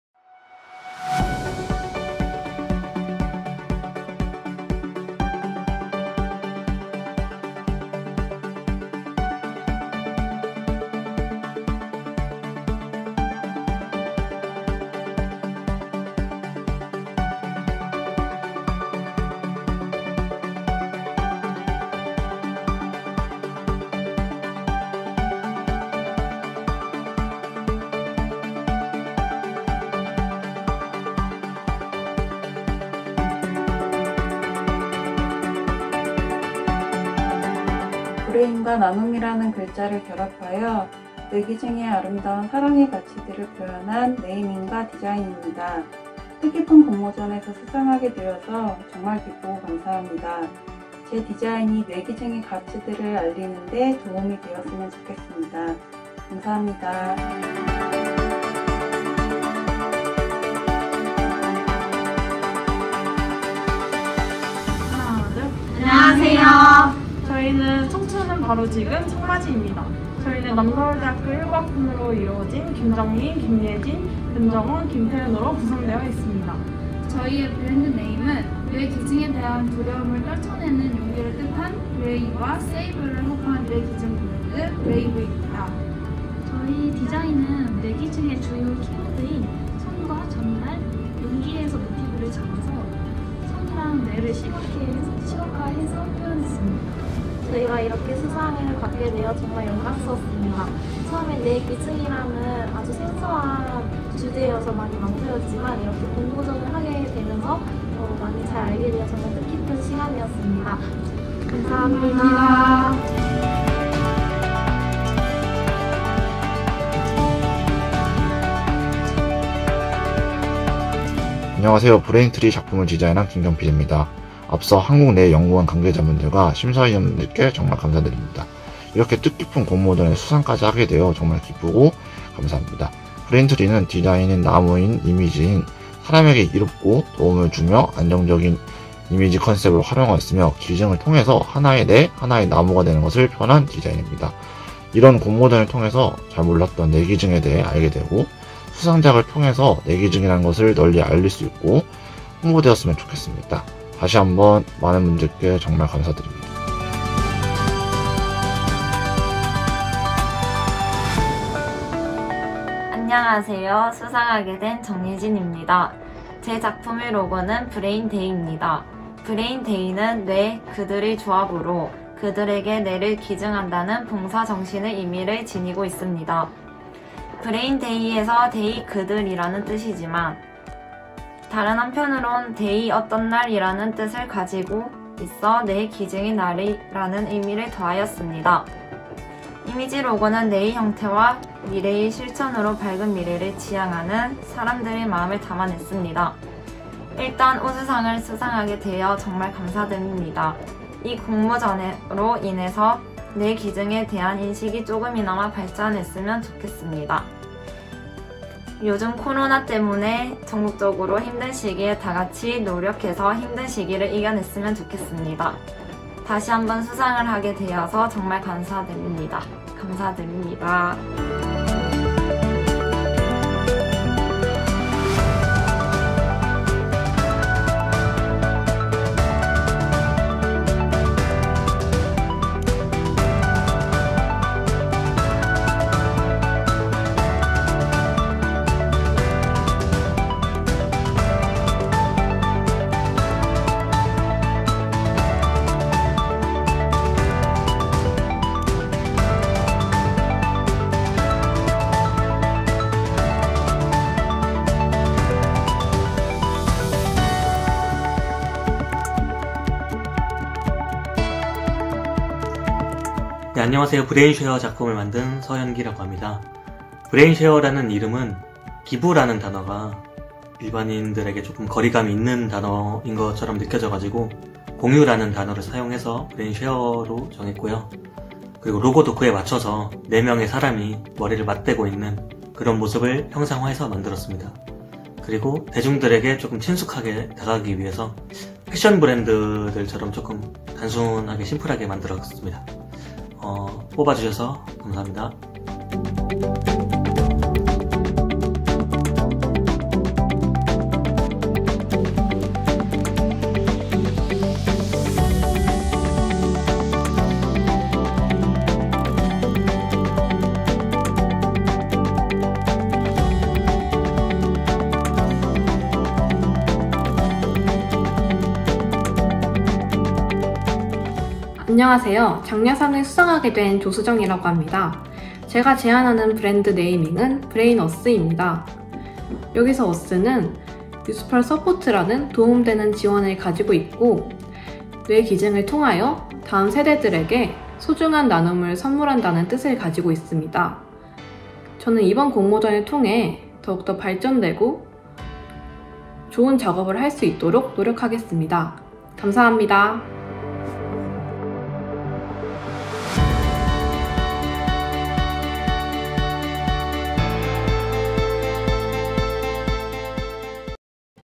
[KBB] 한국뇌은행 (Korea Brain Bank) 뇌기증 홍보 브랜드 공모전 온라인 시상식 영상
안녕하세요.
코로나 여파가 좀 잠잠해지나 싶더니!
오미크론의 여파로 인하여 다시금 ㅠㅠ 힘들어지고 있습니다.
모두 이럴때 일수록 건강 잘 챙기셔야 합니다.

이번 영상은 얼마 전에 진행되었던 한국뇌은행(Korea Brain Bank)에서 진행되었던 뇌기증 홍보 브랜드 공모전에 대한 온라인 시상식 영상을 촬영하게 되었습니다.

이번 공모의 주제는 "홍보 브랜드명과 브랜드 로고 디자인"이었으며,
뇌기증에 대한 막연한 두려움을 해소하고 미래세대에게 뇌건강을 선물하는 우리 모두의 희망 실천임을 알리는 내용입니다.
뇌기증의 숭고한 가치와 존엄성을 고취시킬 수 있는 창의적이고 상징적인 문구에 대해 공모전을 진행 하였습니다.

공모전에 지원해주신 분들께 다시 한번 진심으로 감사의 인사를 올리며,
뇌기증 홍보 브랜드 공모전에 수상하시게 된 분들도 진심으로 축하합니다.!

감사합니다 :)